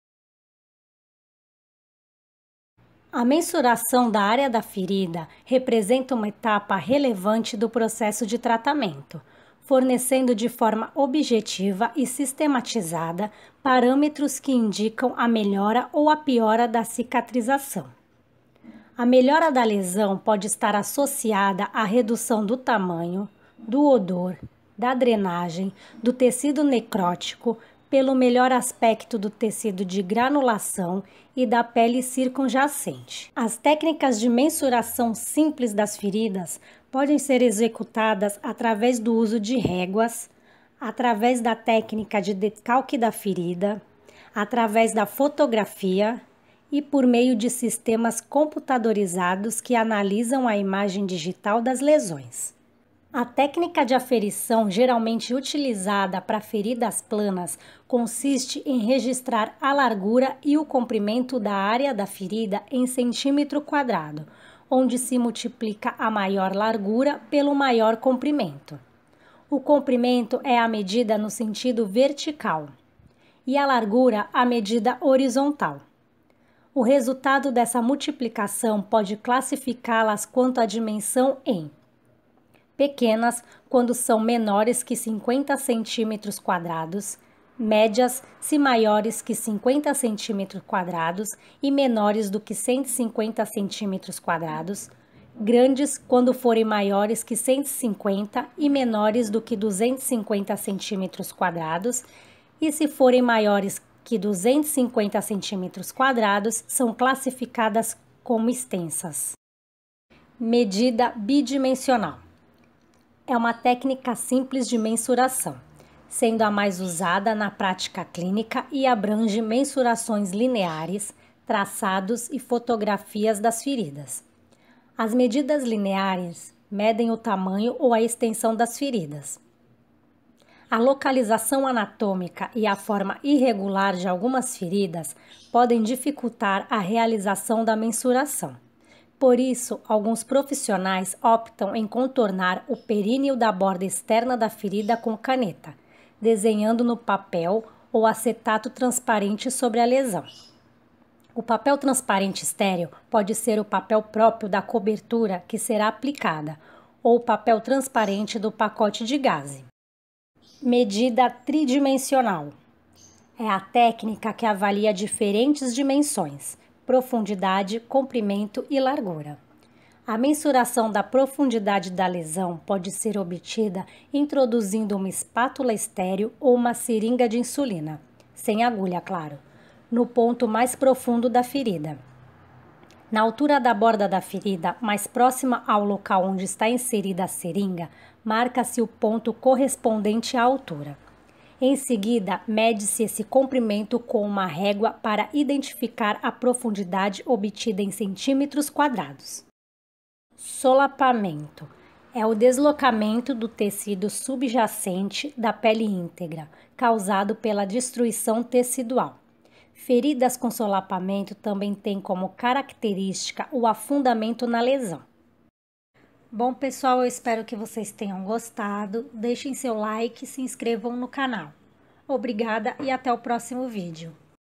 Mensuração da lesão Ferida - Enfermagem EDUCACIONAL
🏥 Descrição:
A pele é o maior órgão do corpo, representa em torno de 15% do peso corporal e tem as seguintes funções: proteção, percepção do ambiente, retenção de água, termorregulação, resposta imunológica, produção de vitamina D e resposta emocional. Quando ocorre o rompimento da estrutura e da função anatômica normal da pele, tem-se uma ferida.
Para a cicatrização destas, ocorre uma sequência de eventos complexos. Uma ferida se torna crônica quando ocorre um retardo nas etapas do processo cicatricial. O profissional de enfermagem está diretamente relacionado ao cuidado a pessoas portadoras de feridas em todos os níveis de atenção à saúde. Por isso, precisa realizar uma boa avaliação clínica.